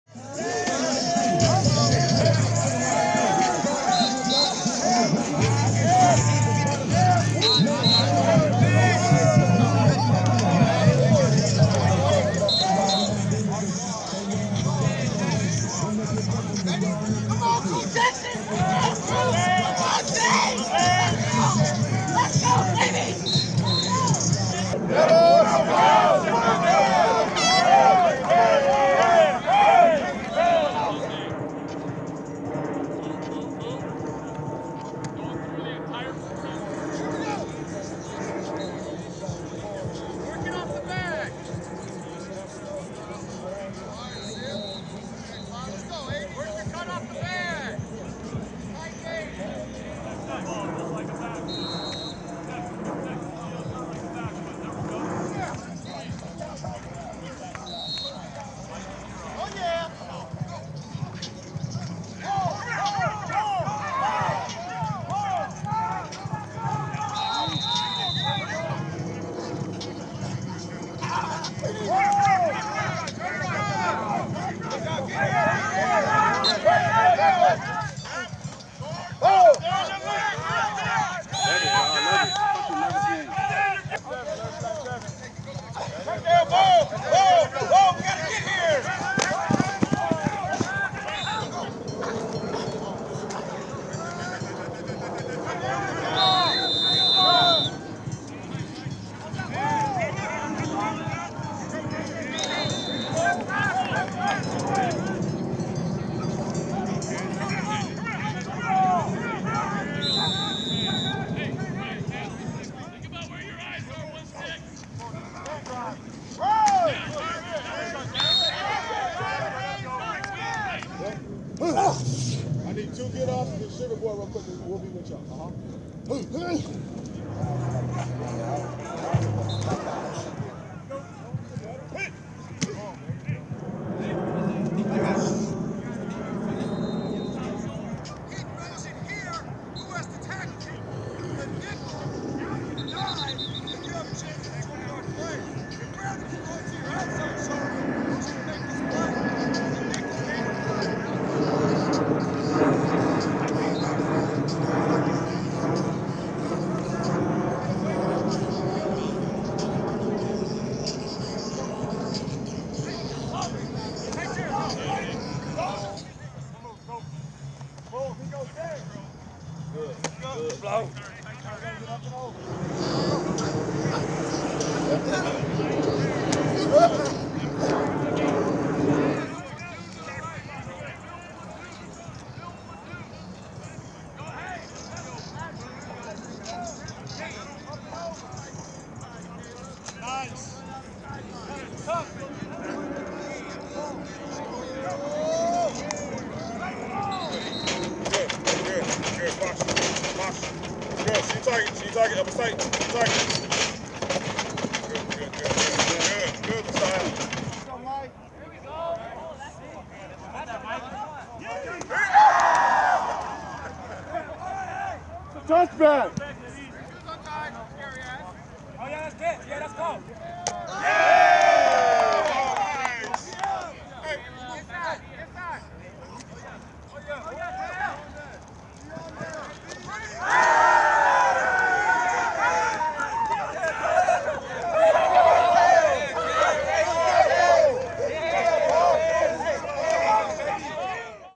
ASU Football: Sights and Sounds 8/23/2023
Jack Pierson captures the best moments from Arizona State's practice in Tempe.